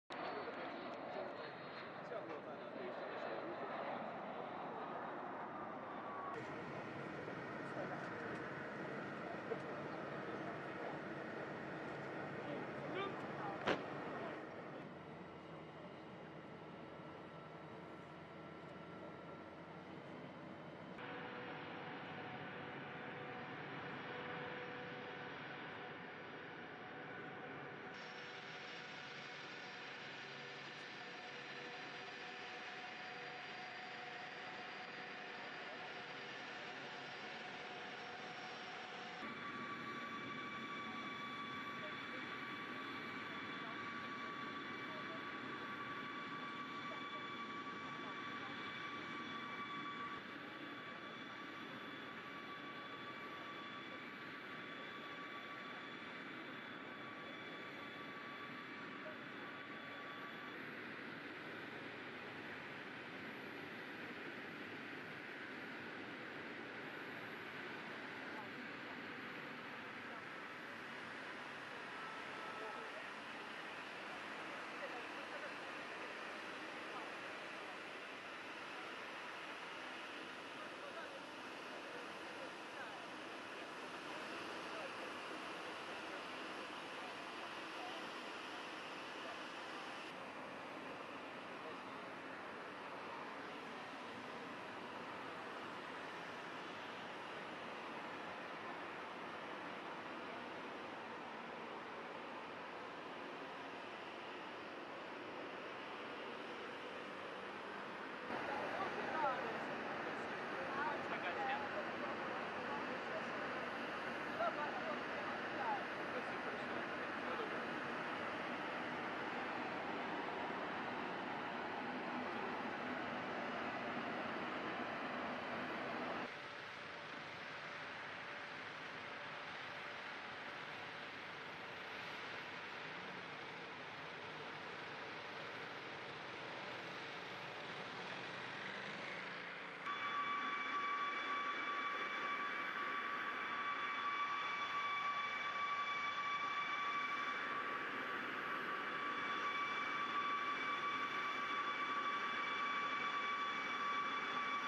PUTIN ARRIVES IN BEIJING ON FIRST FOREIGN VISIT SINCE RE ELECTION
Russian President Vladimir Putin arrived in Beijing on Wednesday for a state visit at the invitation of Chinese President Xi Jinping. Footage shows the presidential plane landing on the runway, where a Chinese delegation and honour guard greet the Russian leader as he disembarks and makes his way to the motorcade.